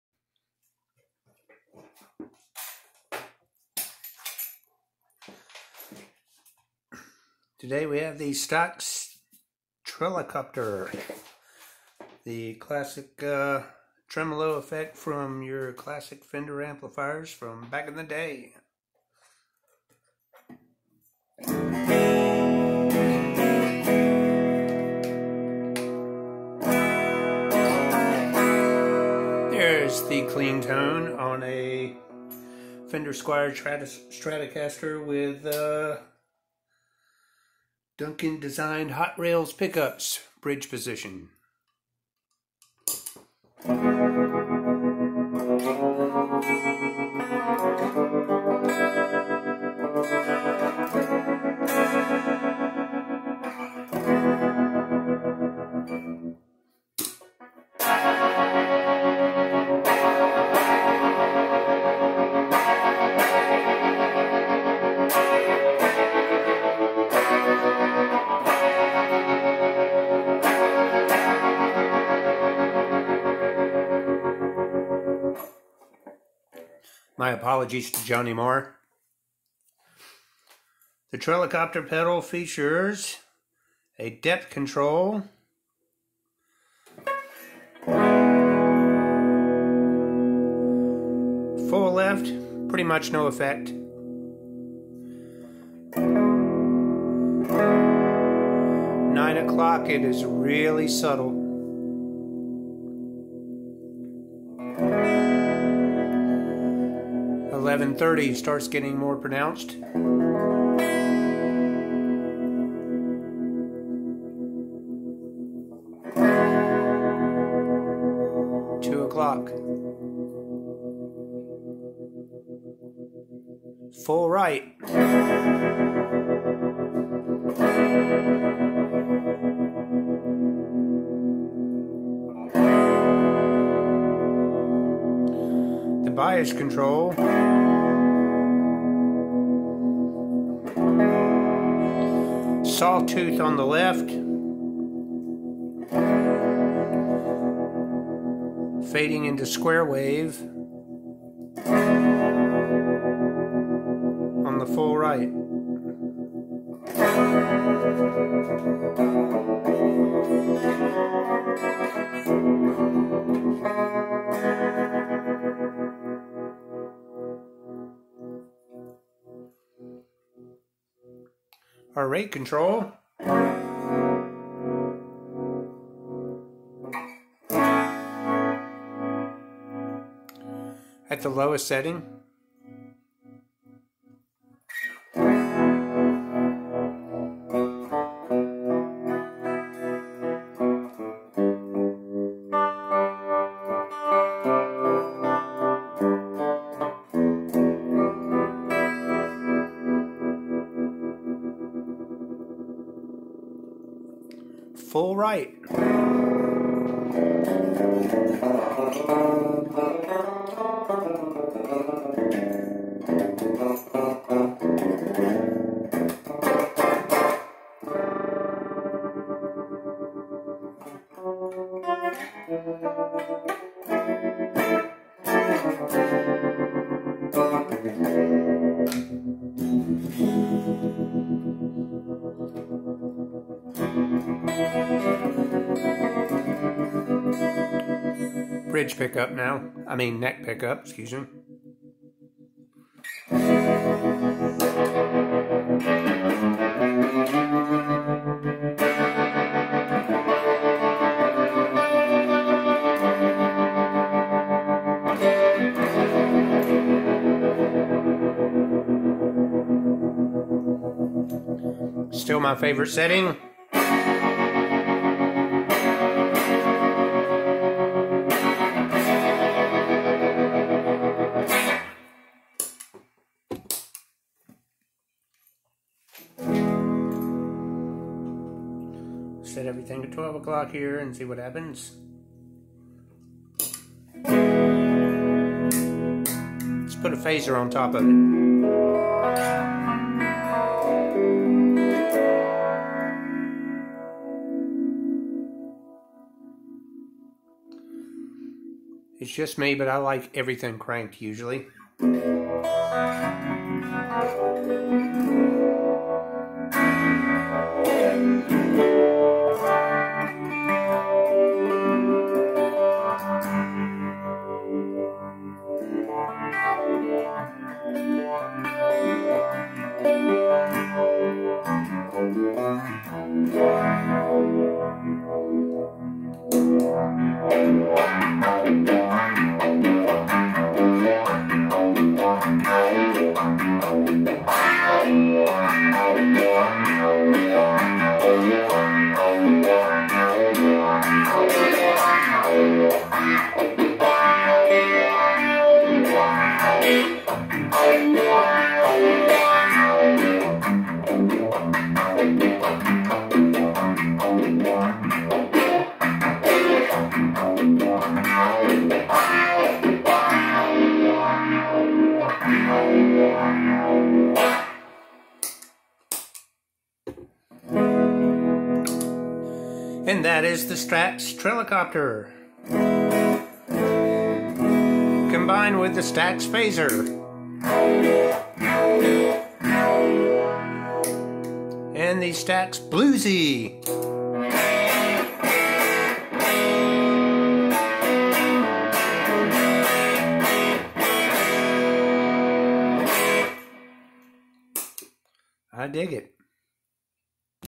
Stax Trelicoptor Tremelo demo
the classic effect found in classic Fender amps from Ye Olde Daye

Stax Guitar Tremolo Pedal...

Stax Guitar Distiortion Pedal...